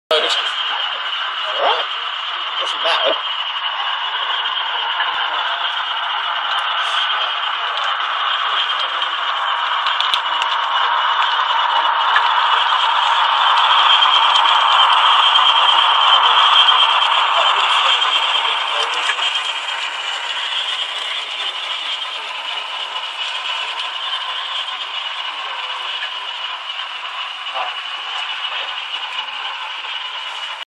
Class 40 no. 211 with snow ploughs
211 trundles through Widnes Road with the ploughs on Paul Smith's layout at Croydon MRS's show on 13th October 2012.